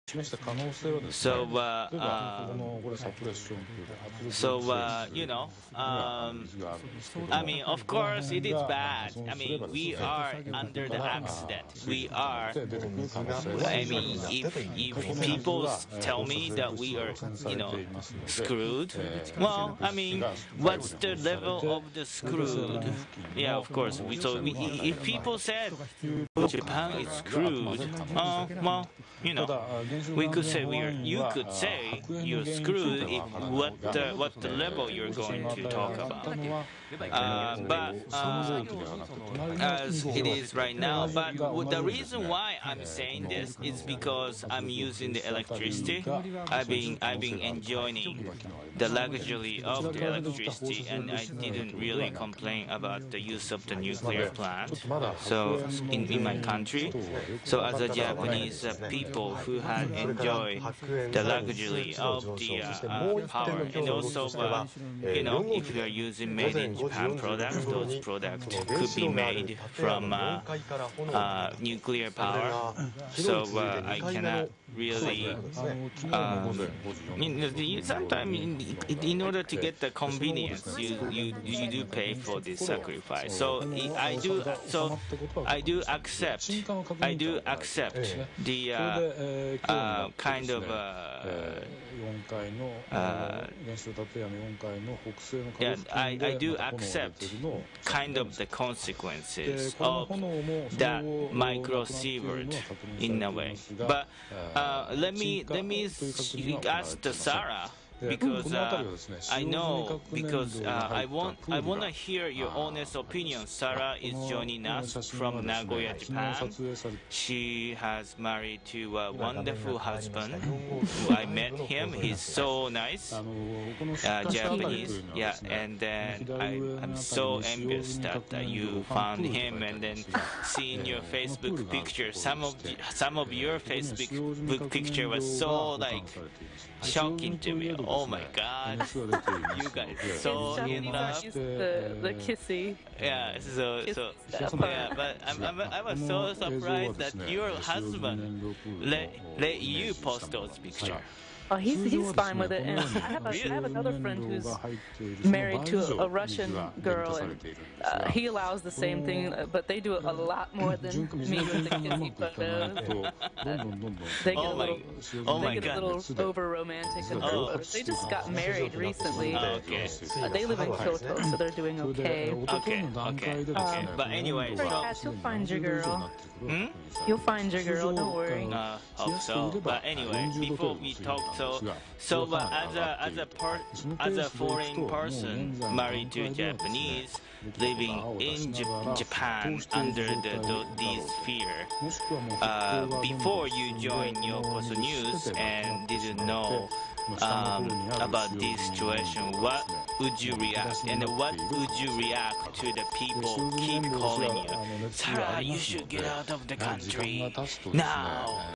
keeping infirmed and current affairs
This link is the best way to stay on top of things as they happen. On the left is a Japanese NHK stream, on the right is the Japanese TBS stream, and in the middle is Katz, who does real-time translation and some analysis. NHK World and the Western media are all "breaking" stories that are hours out of date.